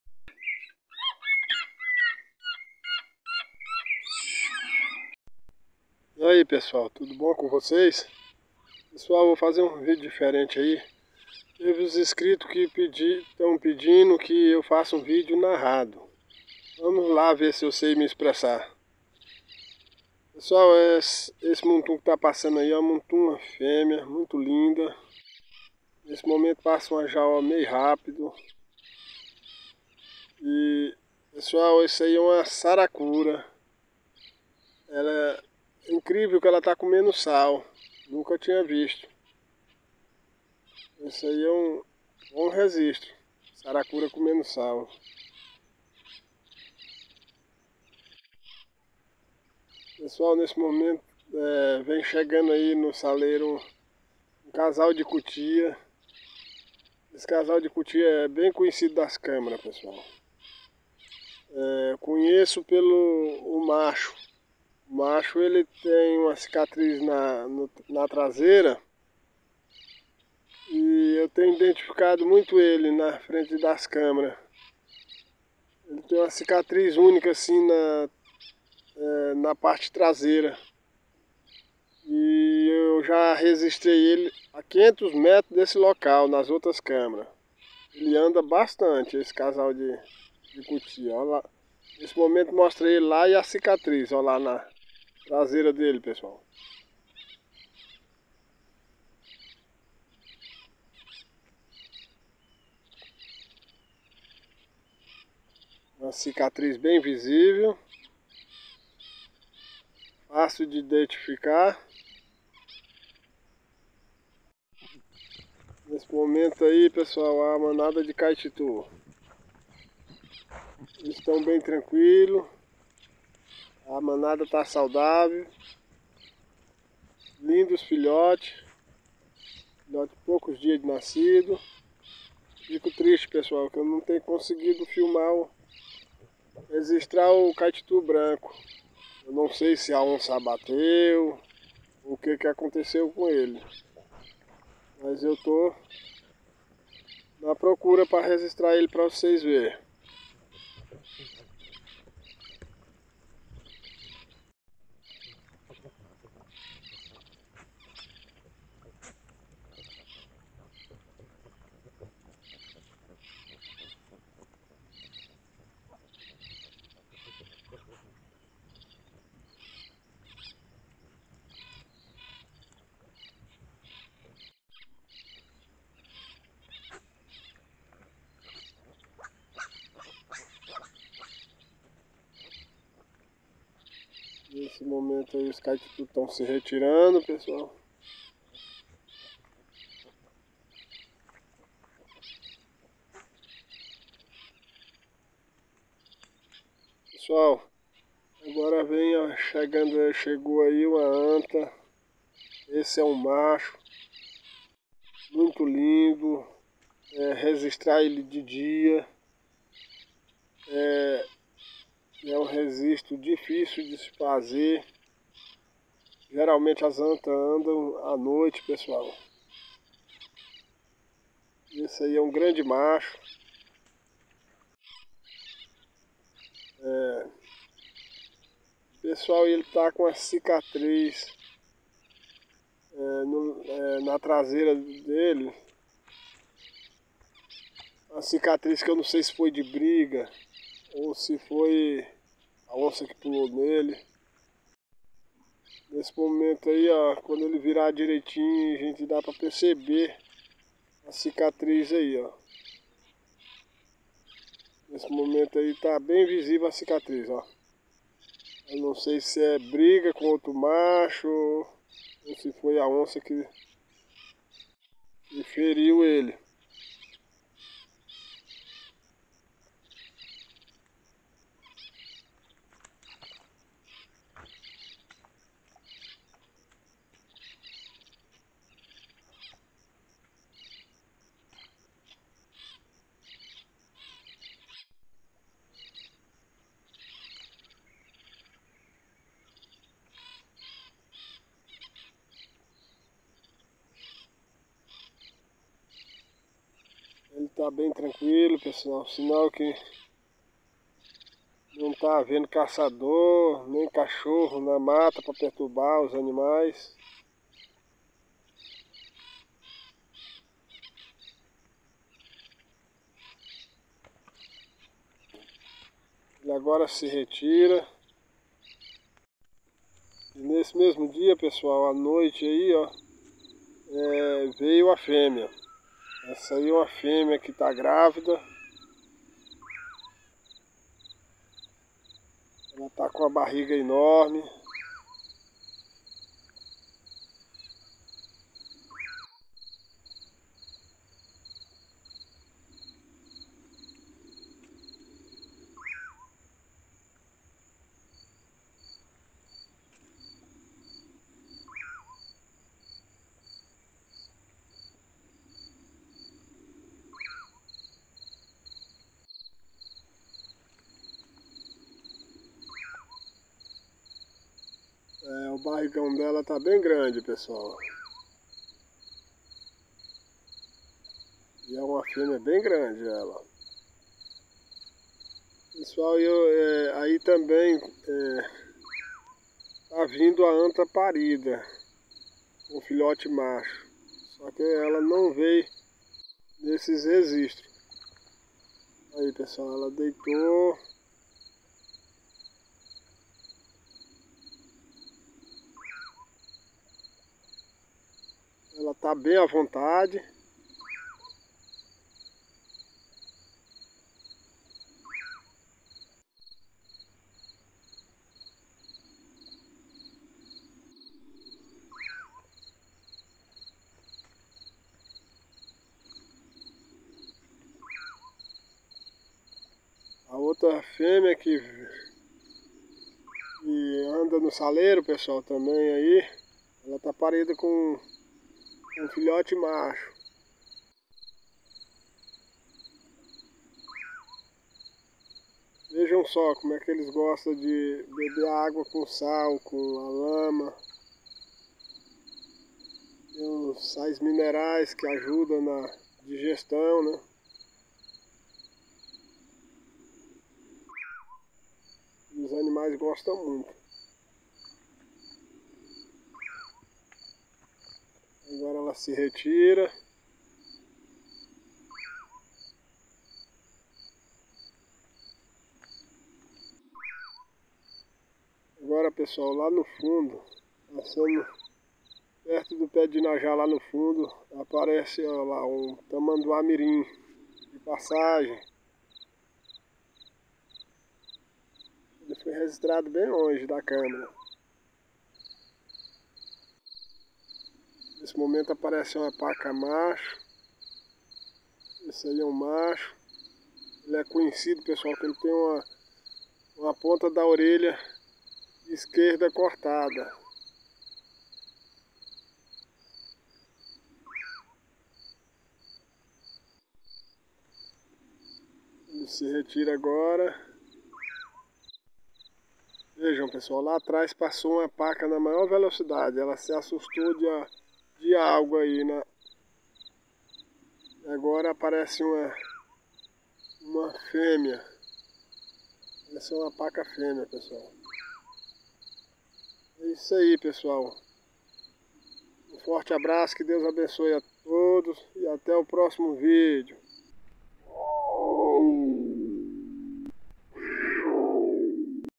Muito top registrei a ANTA em plena luz do dia, e várias espécies, vídeo narrado...
Esse vídeo mostra várias espécies no saleiro, lindo ver a anta de dia vejam.
Devido as câmeras terem áudio baixo ou nem um eu coloco canto de pássaros de nossa fauna de fundo.